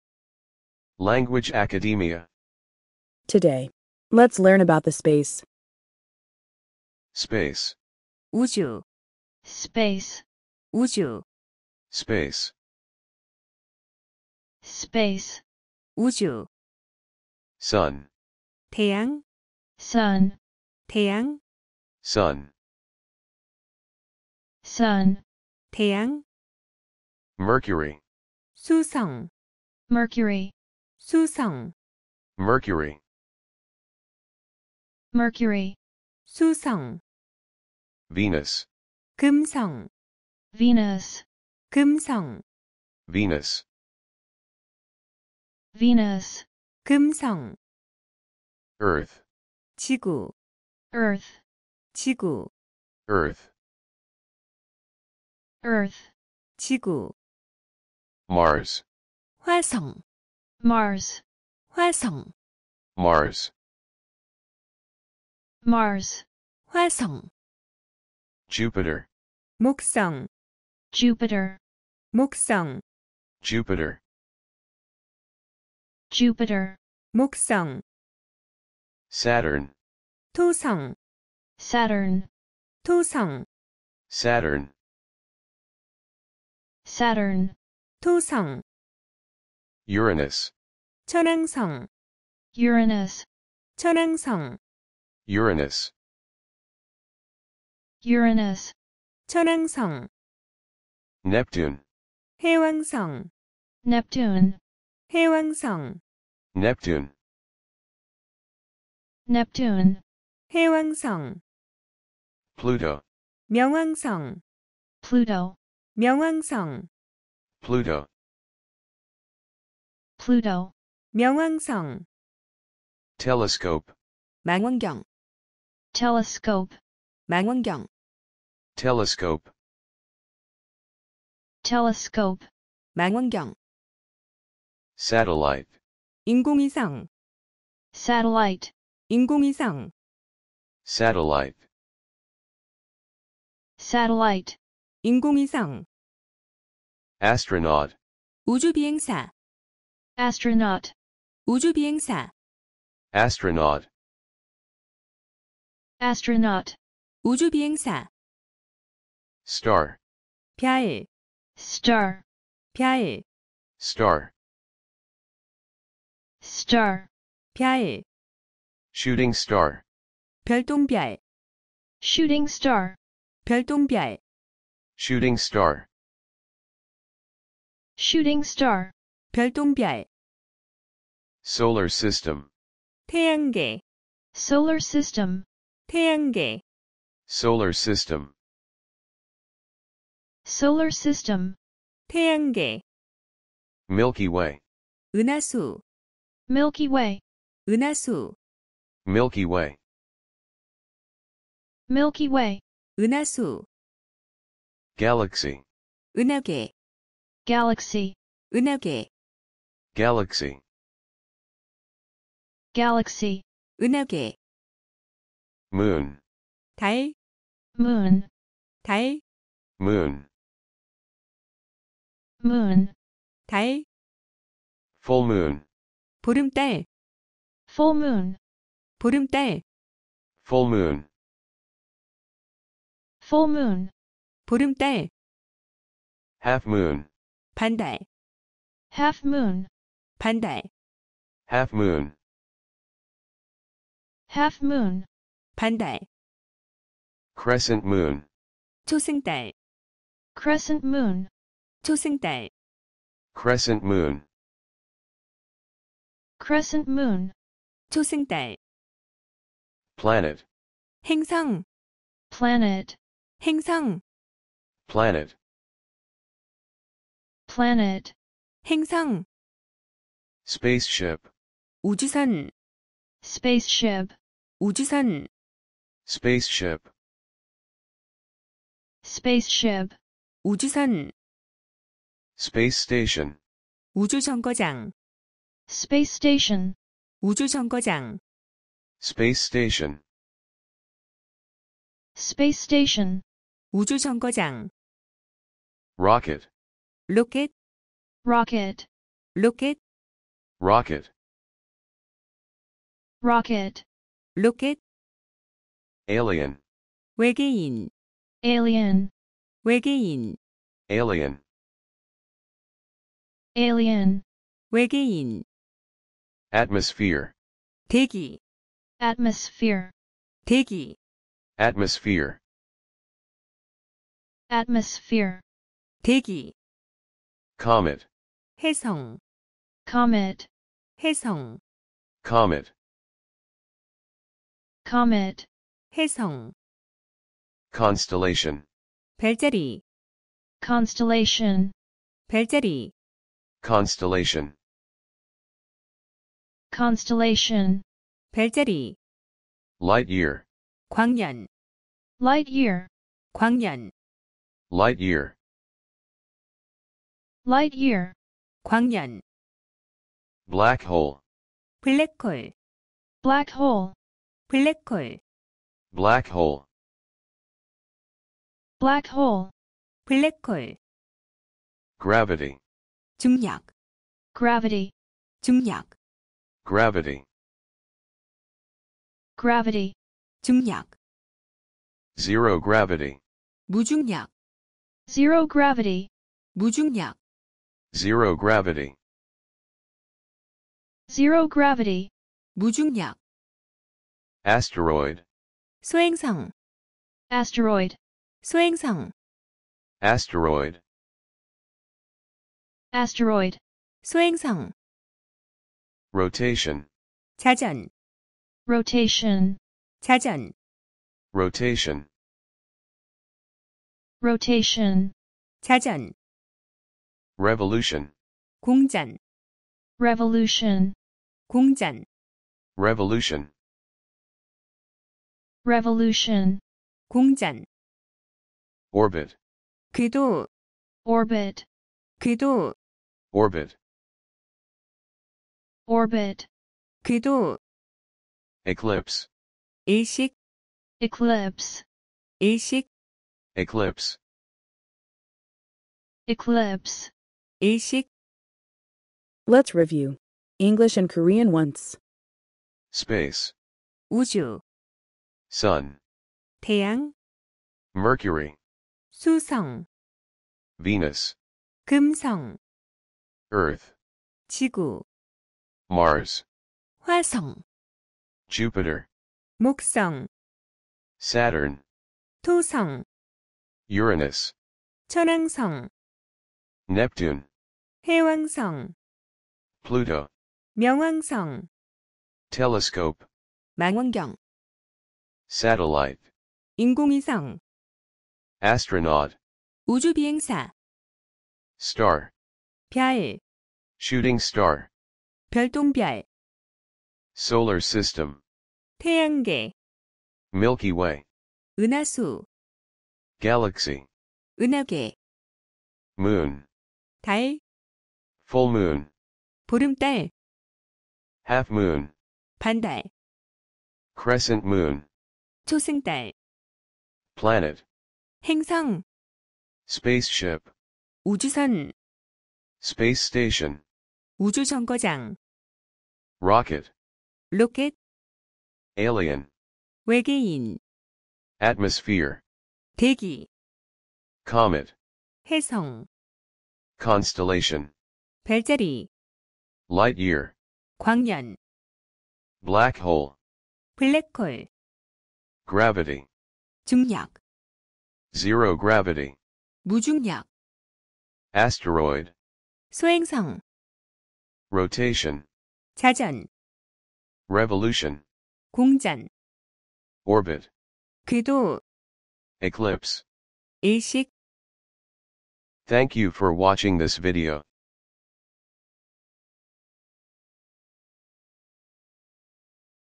아주 쉬운 우주 영어 단어 [우주-SPACE] - 40/ 우주 영어읽기 / 우주 영어듣기 / 영어회화 / 기초영어단어 / 사진과 함께해요! / 초보영어 / 초등영어단어
1) 사진과 함께하는 우주 영어 단어 [우주-SPACE] - 40/ 우주 영어읽기/ Earth, Galaxy, Sun, Orbit...

2) 처음 영어단어를 공부하는 분들에게 유용하고 기본이 되는 영어단어들을 엄선했습니다.

3) 학습 : 영어 3회 + 우리말 3회  [사진과 함께 학습하세요]
     복습 : 영어 1회 + 우리말 1회  [빠르게 복습을 진행합니다]

4) 학습시작 :  00:06 ~
    복습시작 :  07:30 ~ [빠른 영어1회 + 우리말 1회 복습]

감사합니다. ^^